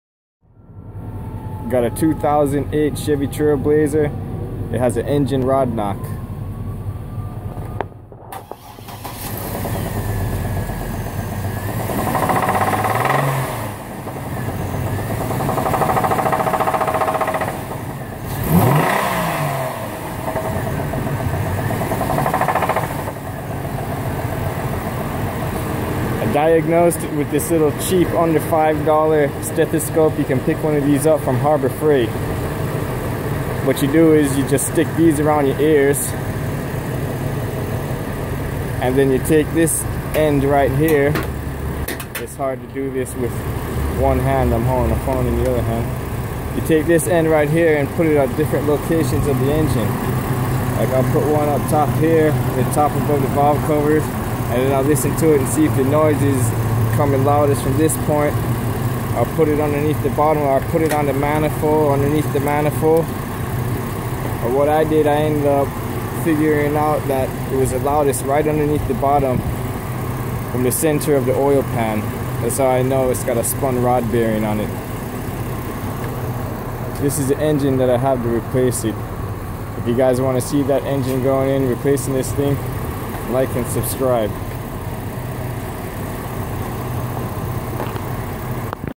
How to diagnose trailblazer engine rod bearing knock
2008 chevy trailblazer with spun rod bearing knocking, diagnosed with cheap harbor freight stethoscope